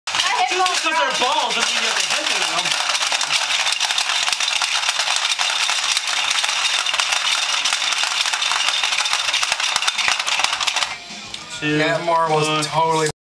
Hungry Hungry Hippos at WSIN
One of the main adventures of my radio show, is playing hungry hungry hippos!